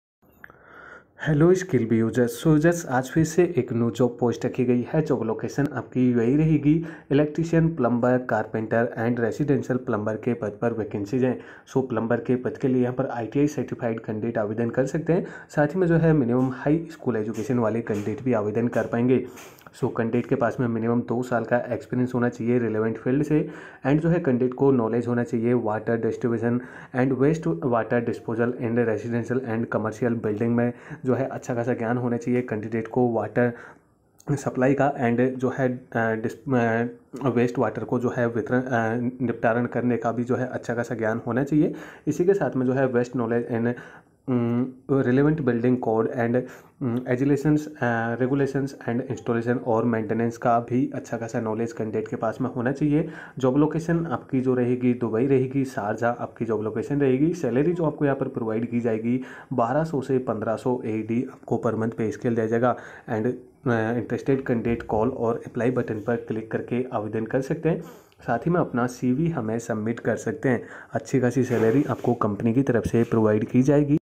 4921 - JOB IN UAE ! Company--GECO Mechanical & Electrical Ltd. Role: Electrician/ Plumber/ Carpenter
4  . DIRECT COMPANY!!  candidates in UAE only!!

Company--GECO Mechanical & Electrical Ltd. Co.

Role: Electrician/ Plumber/ Carpenter, Residential Plumber,

➡️ Location: Dubai / Sharjah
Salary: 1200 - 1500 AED